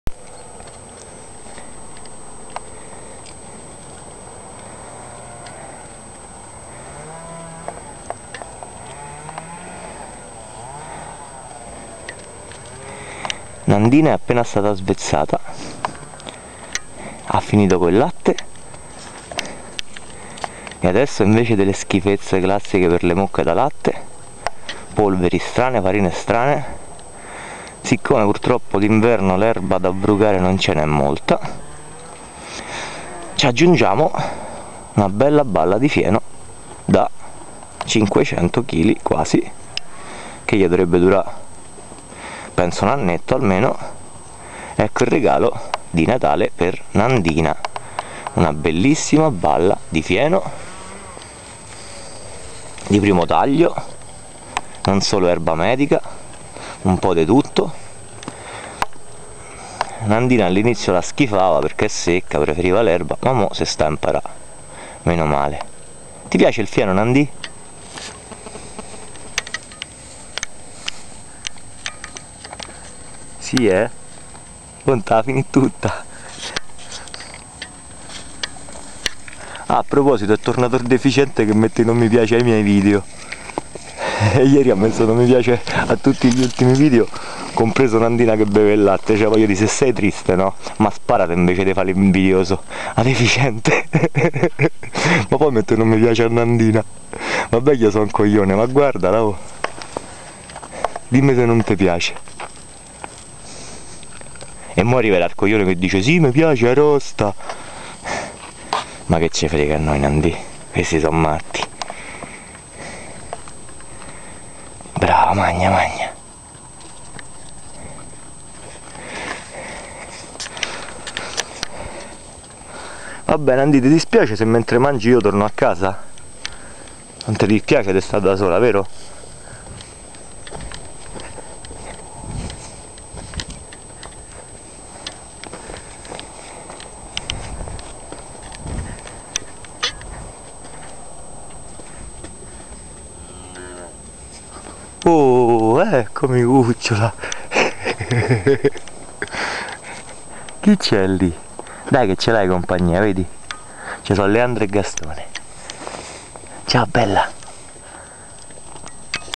Nandina inizia a mangiare il fieno all'ortoddio
Ecco un bel regalo di natale per Nandina.
Quasi cinquecento chili di ottimo FIENO